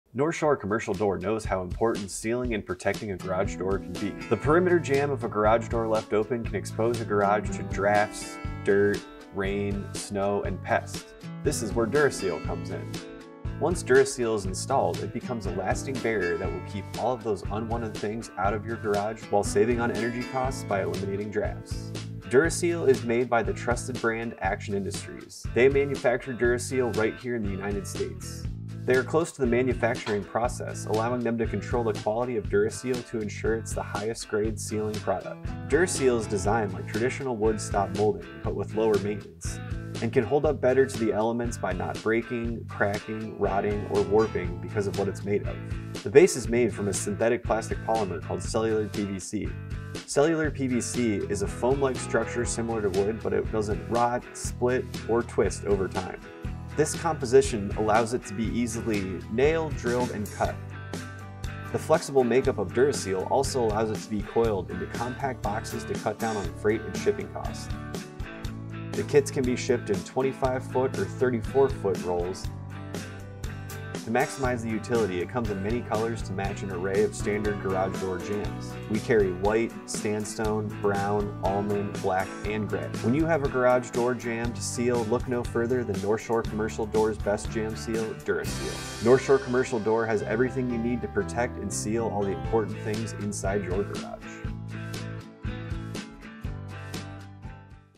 DuraSeal Jamb Seal from North Shore Commercial Door
Sealing a garage door jamb is important for keeping unwanted elements out of a garage and protecting what's inside. This is where North Shore Commercial Door's easy to install jamb seal, DuraSeal comes in. DuraSeal's base is made out of cellular PVC (PolyVinyl Chloride) allowing it to be easily drilled, cut and nailed. This flexible nature allows it to be coiled into boxes to reduce shipping and freight costs. DuraSeal is designed to endure the elements and won't break, crack or rot like conventional wood stop molding. Browse our wide array of of color and size kit options.